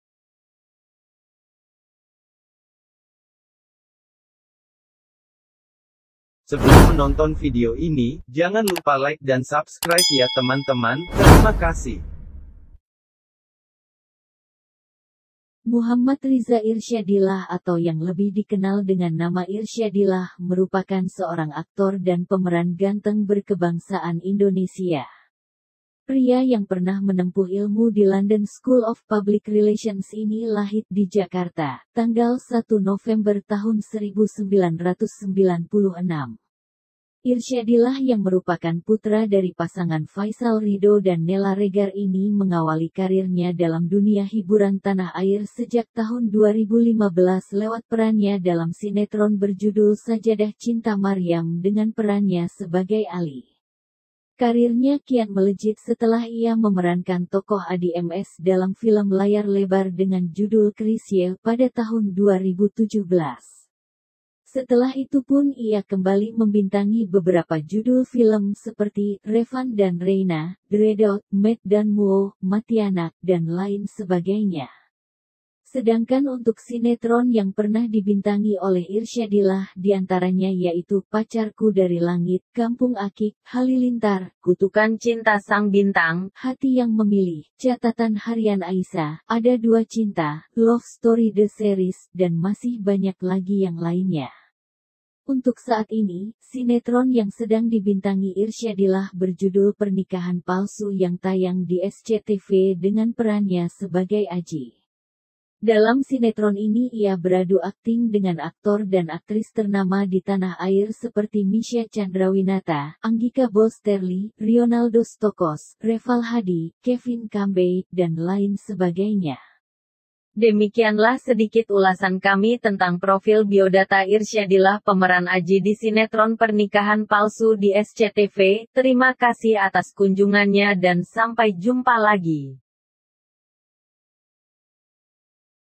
Profil Biodata Irsyadillah Pemeran Aji di Sinetron Pernikahan Palsu SCTV
Muhammad Riza Irsyadillah atau yang lebih dikenal dengan nama Irsyadillah merupakan seorang aktor dan pemeran ganteng berkebangsaan Indonesia. Pria yang pernah menempuh ilmu di London School of Public Relations ini lahid di Jakarta, 1 November 1996. Irsyadillah yang merupakan putra dari pasangan Faisal Ridho dan Nella Regar ini mengawali karirnya dalam dunia hiburan tanah air sejak tahun 2015 lewat perannya dalam sinetron berjudul Sajadah Cinta Maryam dengan perannya sebagai Ali. Karirnya kian melejit setelah Ia memerankan tokoh Addie M S dalam film layar lebar dengan judul "Chrisye" pada tahun 2017.